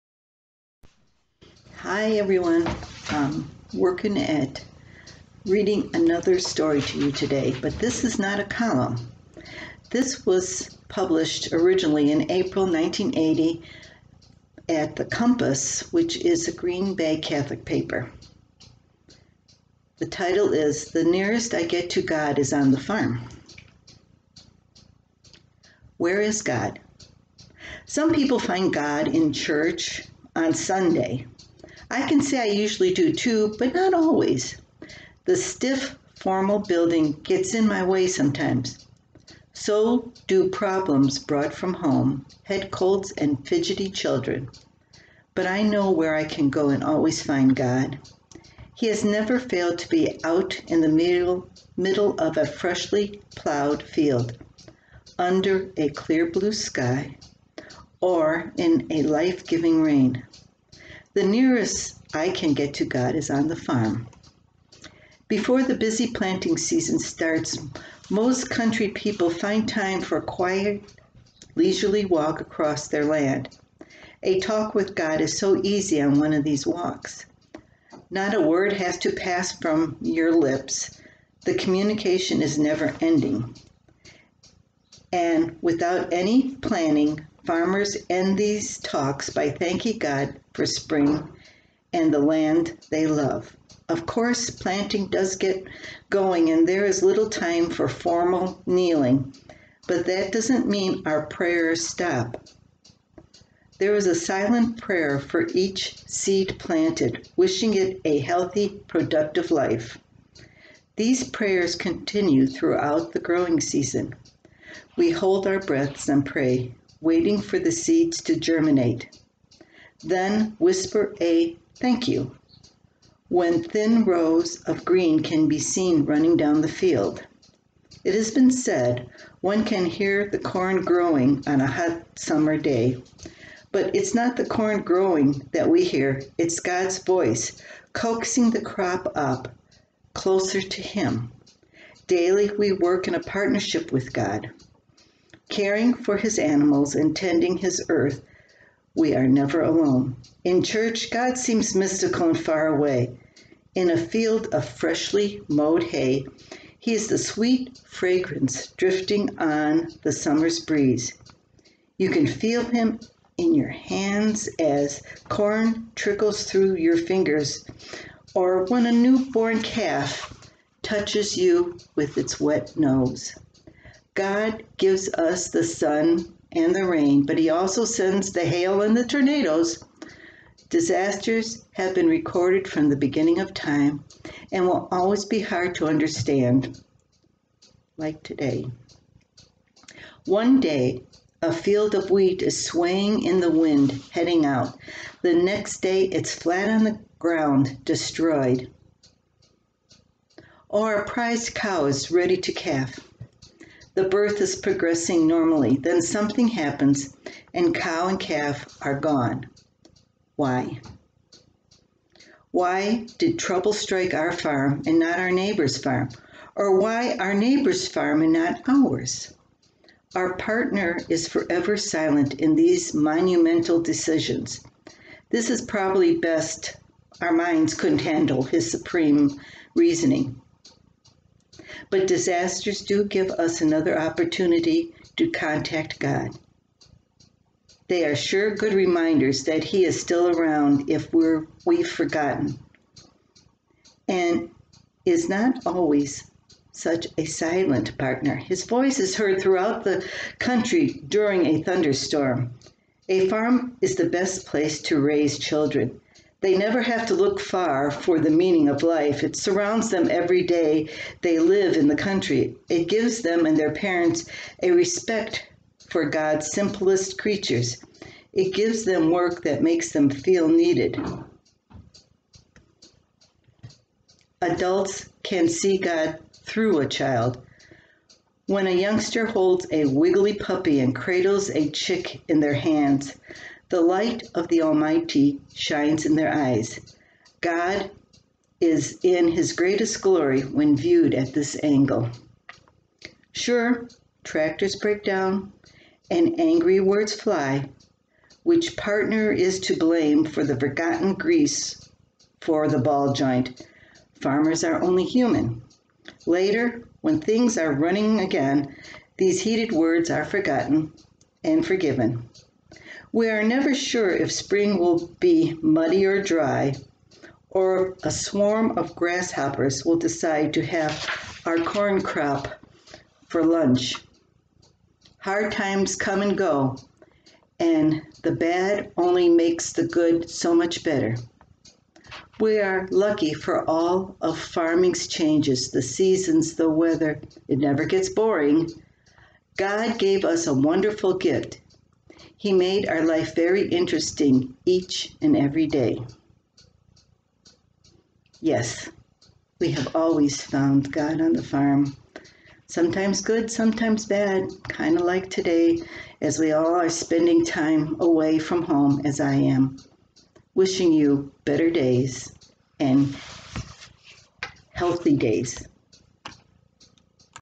Susan Manzke reads her essay, The Nearest I get to God is on the Farm
The nearest I get to God is on the farm.